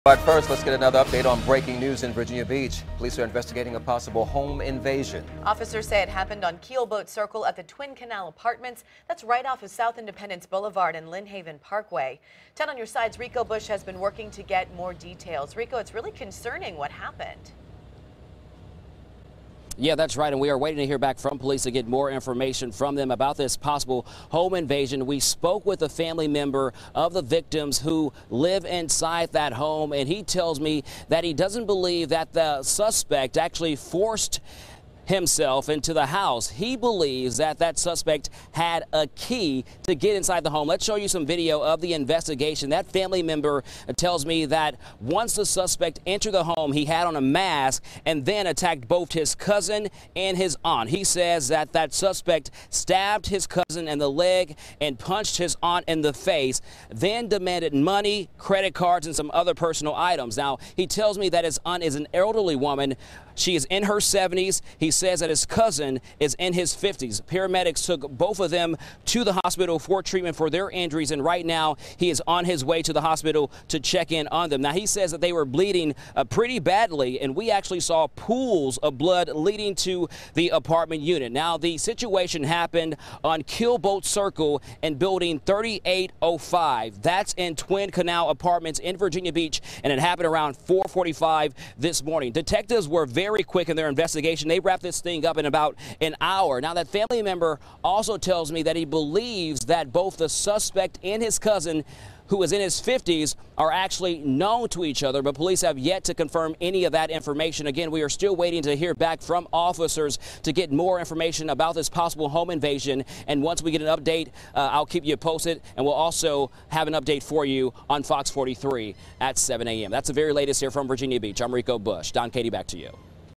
Police investigate reported home invasion in VB
10 On Your Side's Rico Bush reports on the last in a police investigation into a reported home invasion on Thursday, July 14, 2016.